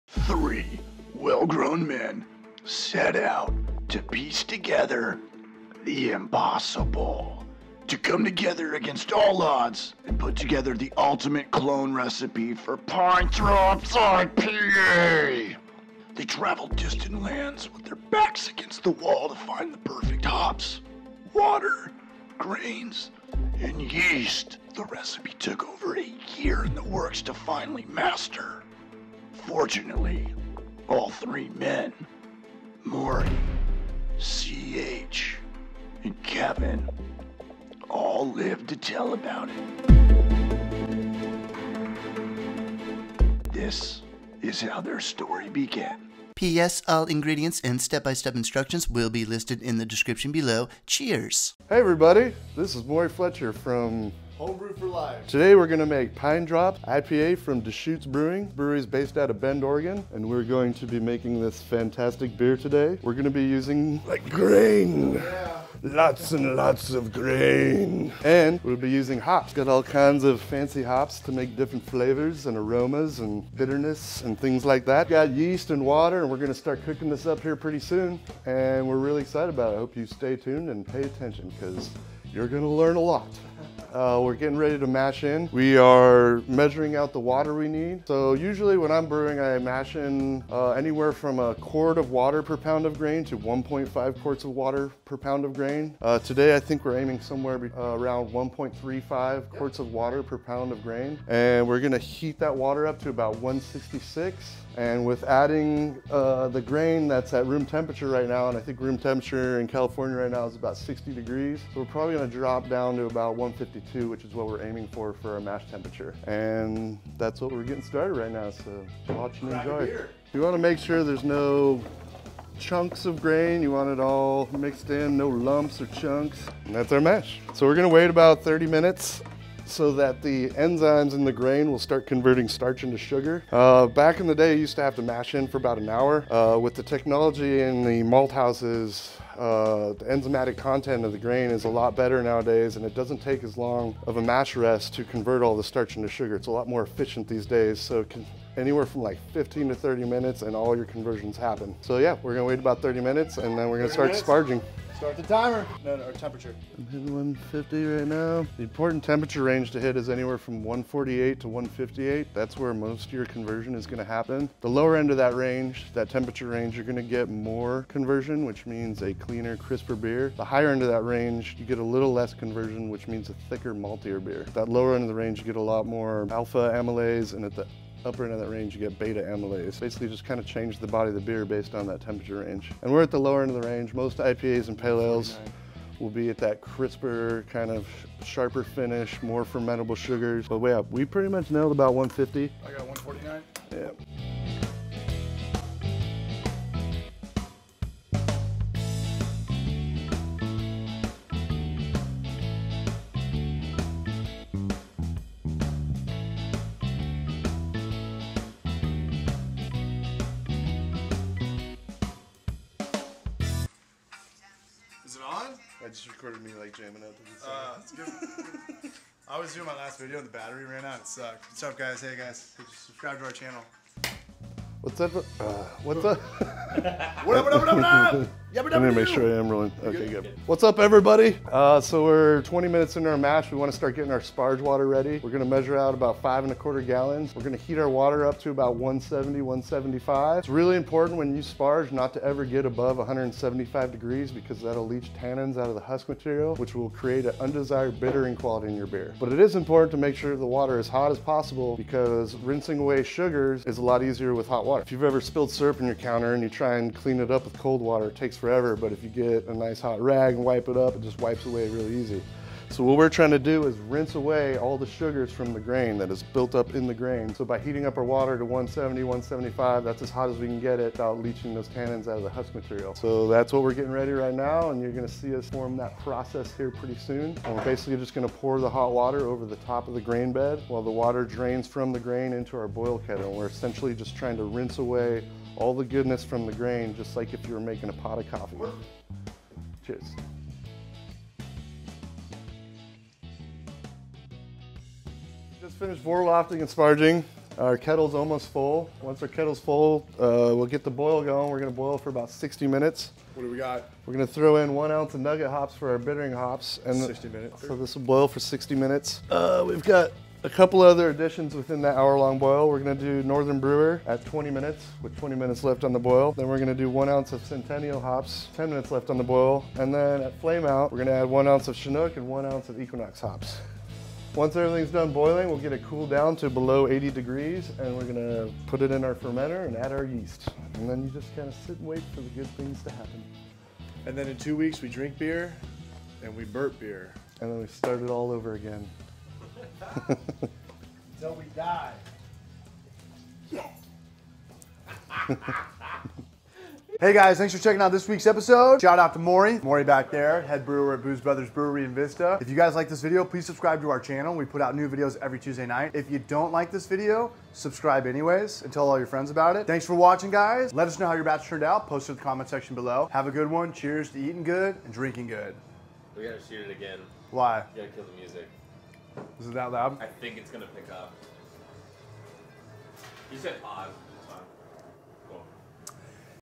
How to All Grain Homebrew PINEDROPS IPA Recipe | Deschutes Brewery
DESCHUTES PINEDROPS IPA:
6.5% abv
70 ibu
OG: 1.064
FG: 1.014

BATCH SIZE:
5.5 gallons

VARIABLES:
grain lbs: 13 lbs
mash thickness (qts/lb): 1.35
target mash temp: 149-152°F
total water needed: 9.63 gallons
mash water needed: 4.40 gallons
sparge water needed: 5.25 gallon
strike temperature: 166°F
sparge temperature: 170-175°F

GRAIN:
12 lbs 2 row
1 lbs: Munich 10L
1 lbs: Carapils (Dextrine)

HOPS:
1 oz nugget 60 min
1 oz northern brewer 20 min
1 oz centennial 10 min
1 pinch irish moss 10 min
1 oz chinook 0 min
1 oz equinox 0 min

DRY HOP:
2 oz chinook 7 days
1 oz equinox 7 days